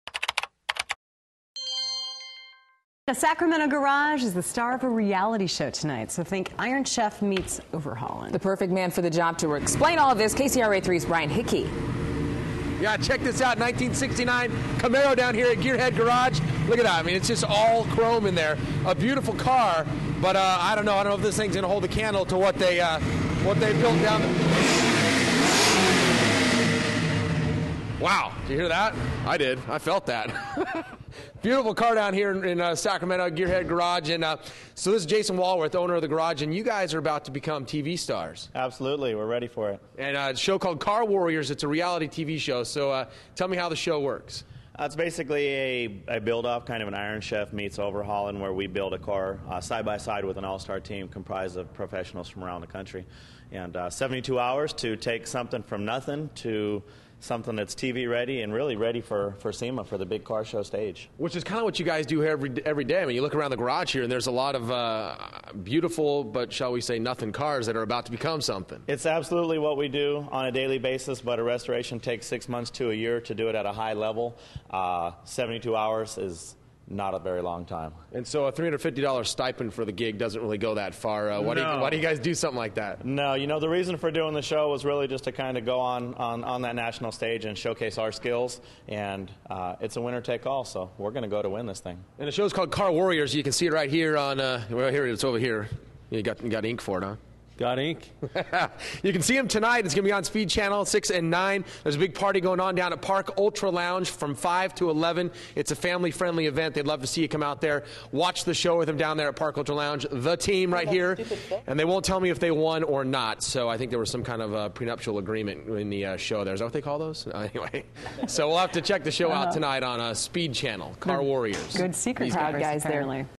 Sacramento Garage Stars In Reality Show
Brian Hickey motors to Gearhead garage in Sacramento, featured in the reality TV show "Car Warriors" on the Speed Channel.